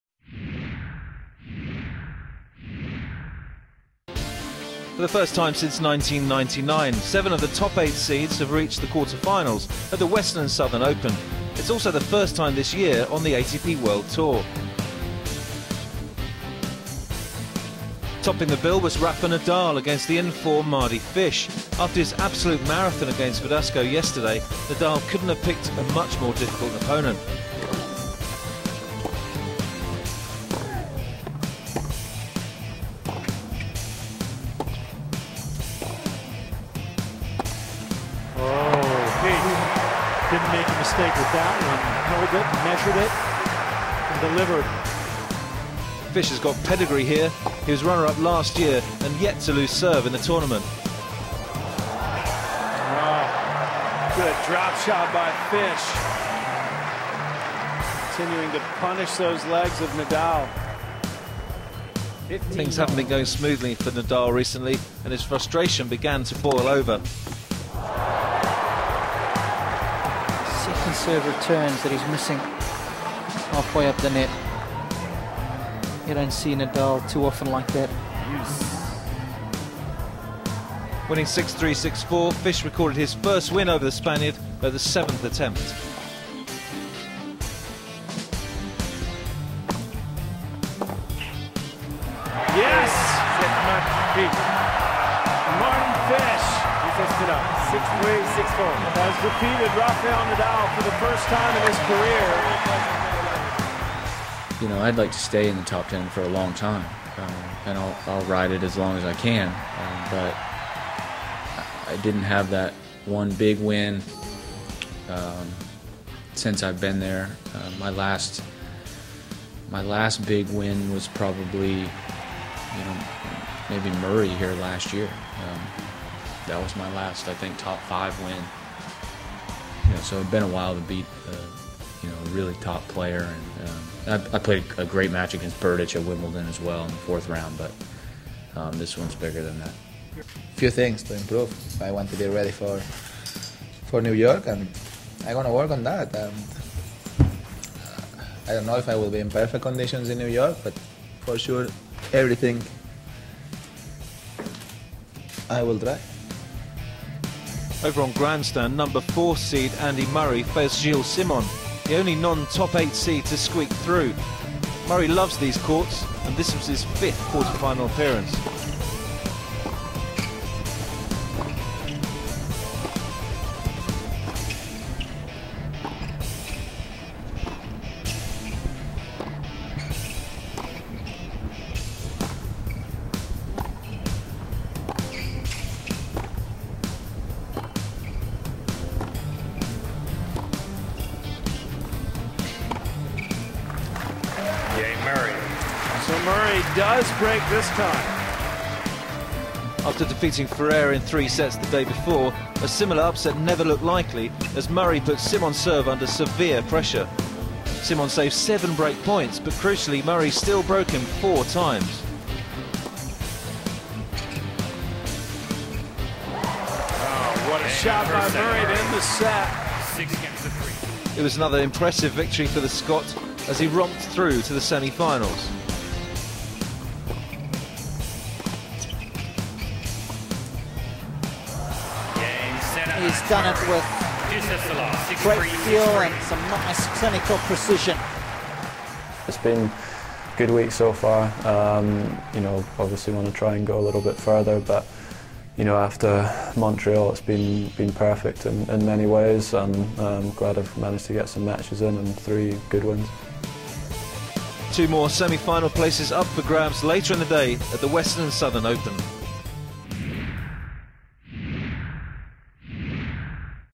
Mardy Fish Upsets Nadal In Cincinnati Quarter-final Highlights
Watch highlights from Friday at the Western & Southern Open, featuring quarter-final victories for Mardy Fish, who recorded his first win against Rafael Nadal, and Andy Murray, who defeated Gilles Simon.